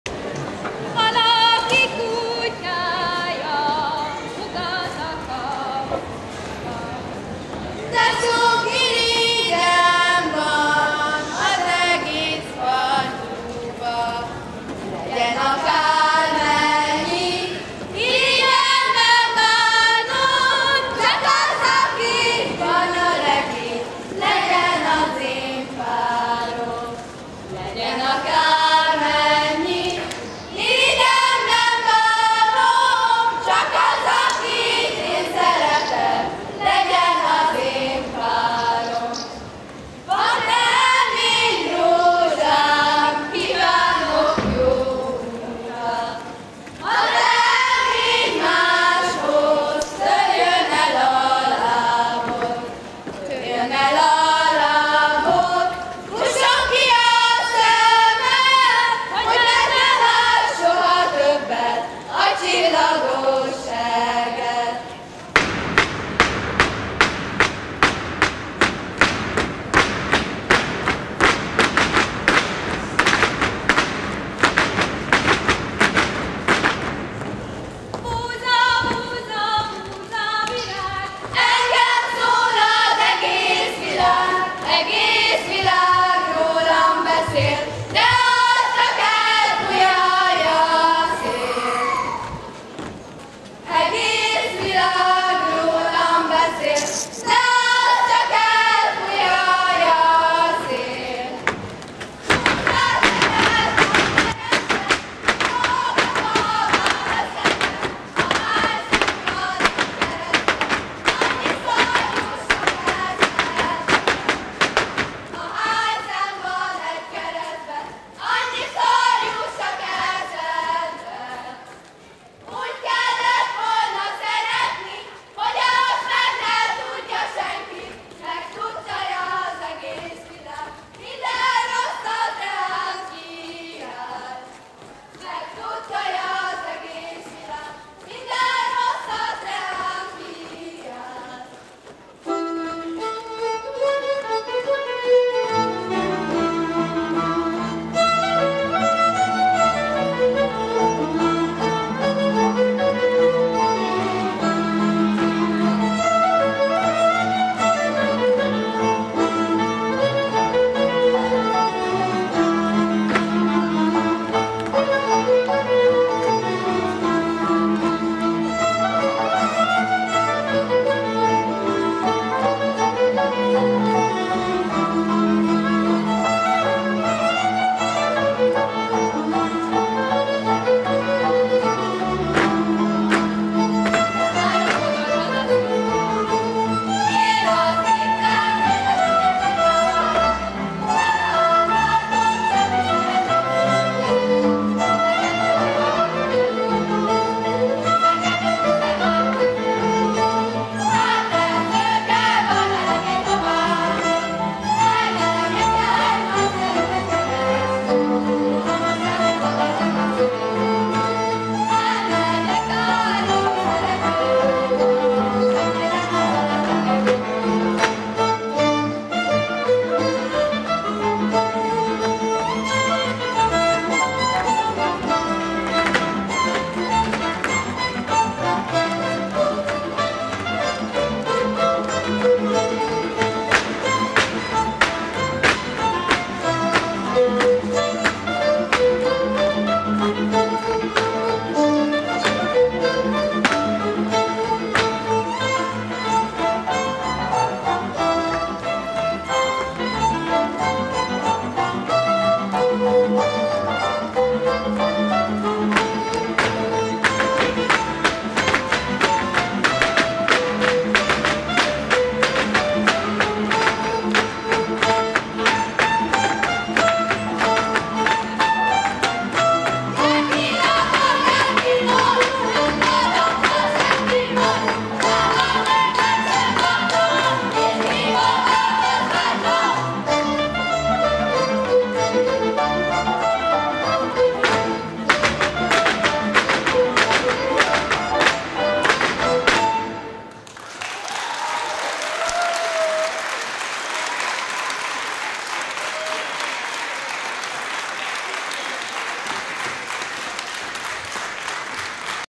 Barackvirág Biatorbágy - Magyarbődi tánc
Vezetők Strack Orsolya, Juhász Sándor. Kísér a Méta zenekar.
XVIII. Méta Fesztivál, azaz Gyermek és Ifjúsági Néptánccsoportok Koreográfia Versenye és Futó Méta Bajnokság.
A Nemzeti Művelődési Intézet és a „Helyiérték" Gondolkodók és Alkotók Regionális Egyesülete szervezésében, a Nemzeti Kulturális Alap támogatásával.
Balatonboglár, 2014. június 7-9. Urányi János Sport és Szabadidőközpont.
Megosztás a szervezők engedélyével (Szalai Tünde).